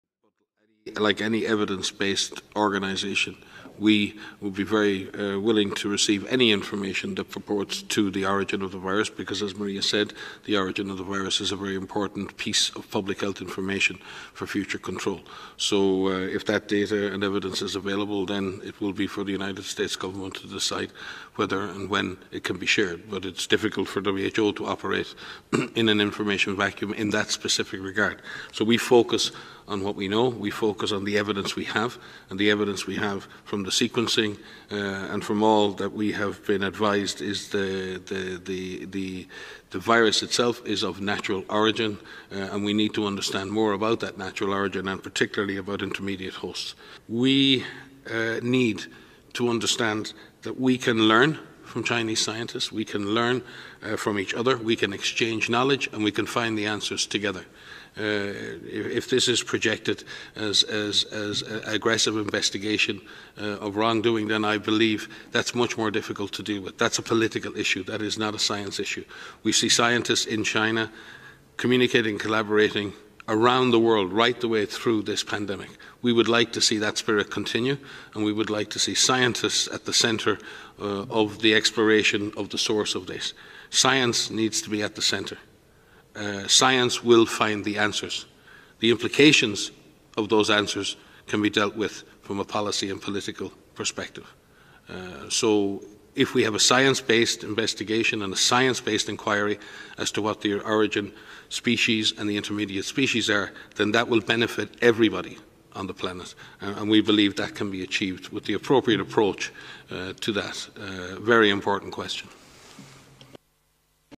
WHO calls for 'scientific-based' inquiry into virus origin
Dr Mike Ryan, the World Health Organization's top emergencies expert on Monday emphasized the need for a science-based inquiry into the origin of the coronavirus.